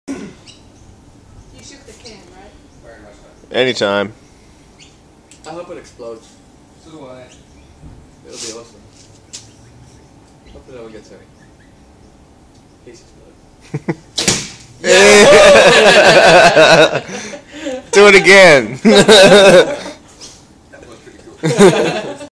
Took an arrow to the can
Shooting an arrow through a soda can using a compound bow
with Tarrion